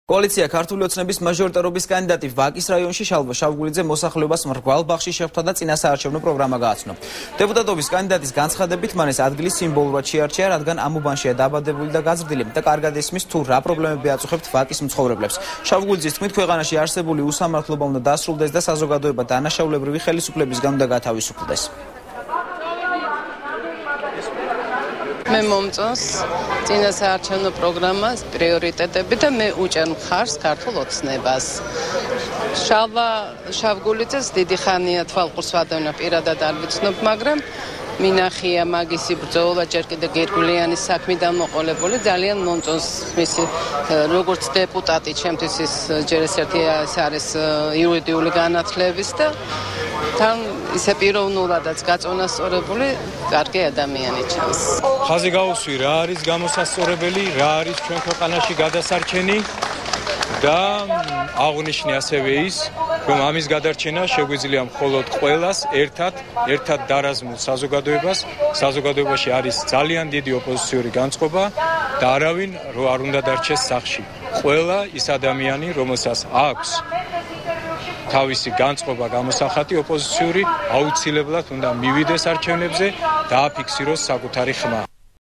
ახალი 12 | შეხვედრა მოსახლეობასთან | 15.08.12
კოალიცია "ქართული ოცნების" მაჟორიტარობის კანდიდატი ვაკის რაიონში შალვა შავგულიძე მოსახლეობას მრგვალ ბაღში შეხვდა და წინასაარჩევნო პროგრამა გააცნო. დეპუტატობის კანდიდატის განცხადებით მან ეს ადგილი სიმბოლურად შეარჩია, რადგან ამ უბანშია დაბადებული, გაზრდილი და კარგად ესმის თუ რა პრობლემები აწუხებთ ვაკის მცხოვრებლებს. შავგულიძის თქმით ქვეყანაში არსებული უსამართლობა უნდა დასრულდეს და საზოგადოება დანაშაულებრივი ხელისუფლებისაგან უნდა განთავისუფლდეს.